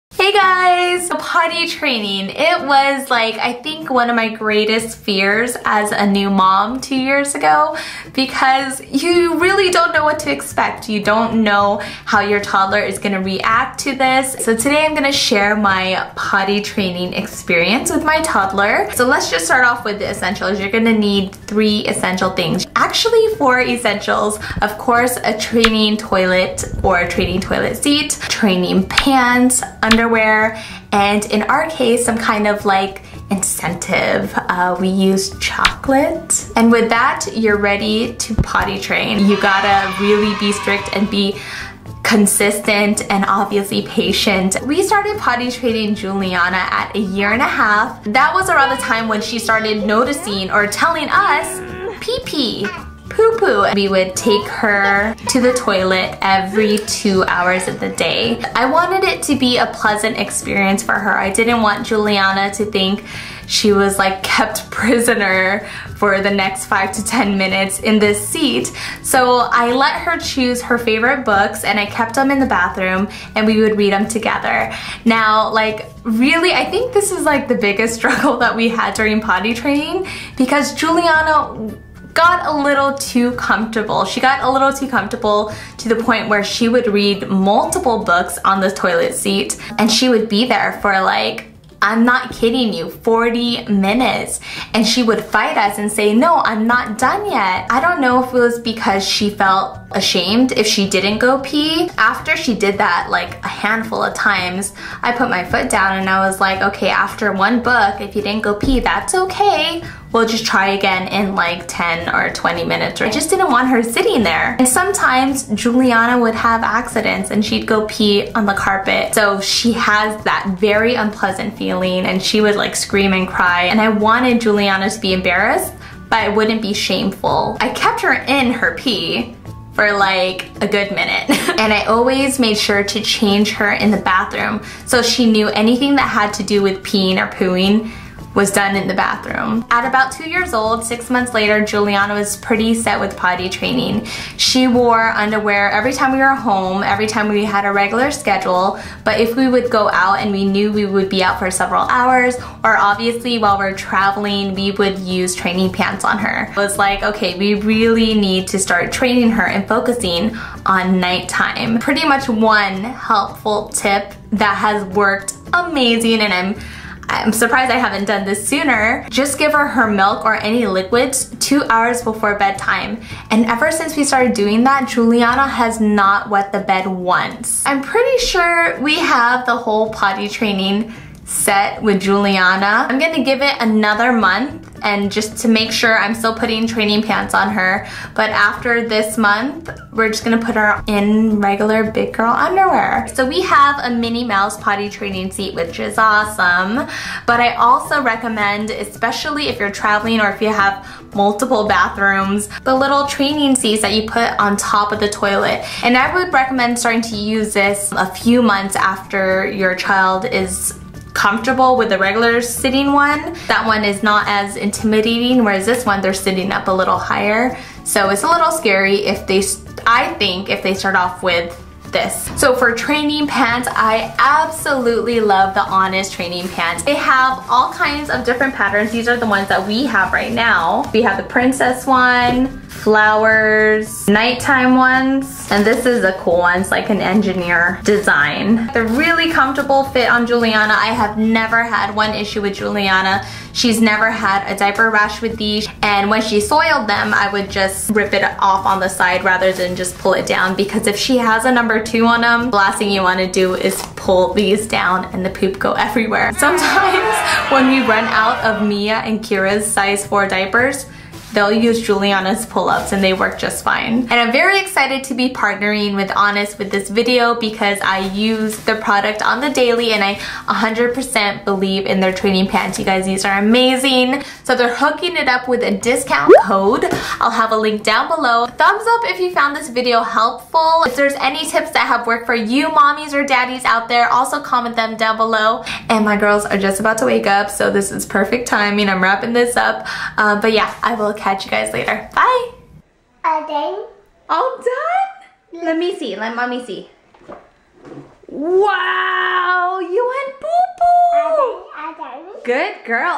How I Potty Trained My Toddler - itsMommysLife
Here's our potty training story, and some tips that helped us through the process!

Itsjudytime in Spanish!